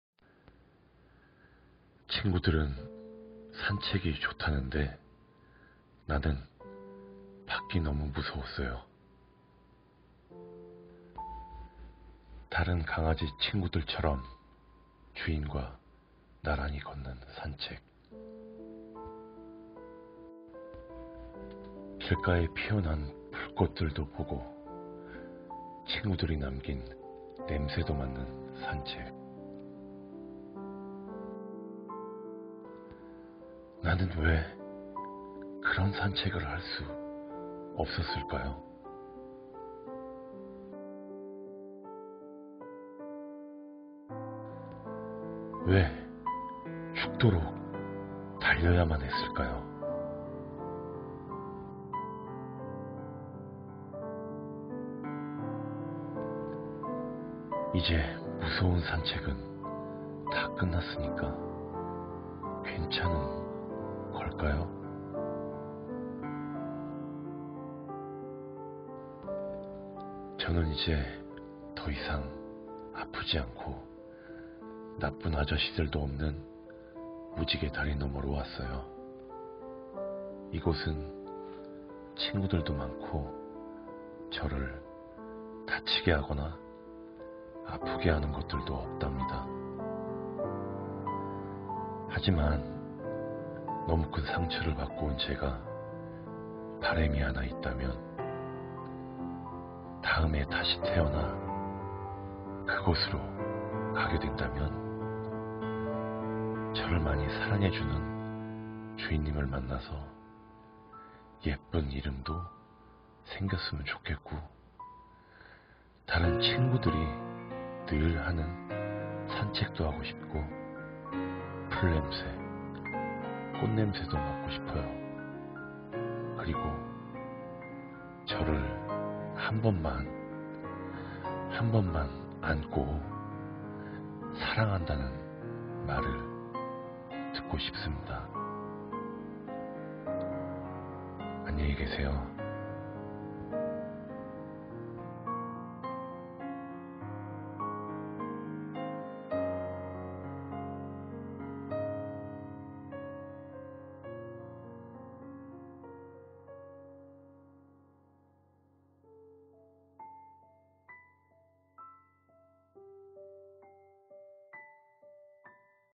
천국이를 잊지않기 위해 만든 이야기
차에 줄이 묶인 채로 끌려가지 않기 위해 죽도록 뛰어야 했고
결국 그렇게 끌려가다가 처참히 눈감아야했던
경북 상주 도로 위의 강아지 천국이

단 한번 산책도 못해보고 풀냄새 꽃향기도 못맡아보고
죽어간 그 연약한 아이를 위해서도 다시는 이런일이
일어나지 않도록 해당 사건 가해자에게
엄중한 처벌이 이루어져야 합니다.

I had to run to death in order not to be dragged with a rope tied to the car.
In the end, I was dragged and I had to close my eyes terribly
Dog Heaven on Sangju Road, Gyeongbuk

I can't even take a walk once, I can't smell the grass and flowers
Even for the fragile child who died, this will never happen again.
To the perpetrator of the incident so that it does not happen.
Severe punishment must be made.